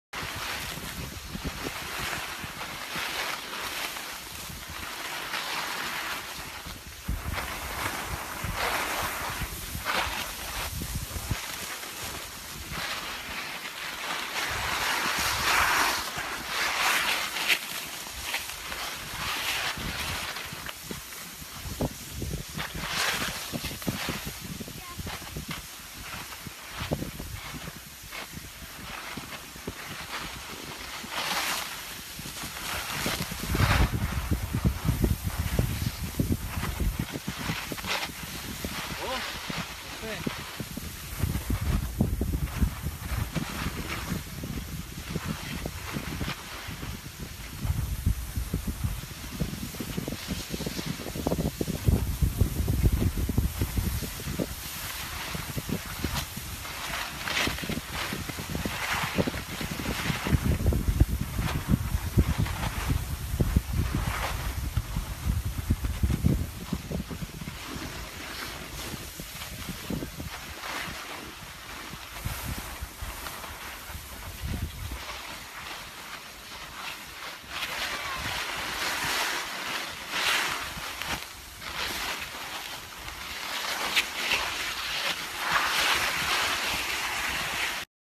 Yüksel SAATCI Bansko Kayak Merkezi 2017
Mavi Pistlerde , Tehlikesiz ve Keyifli Kayak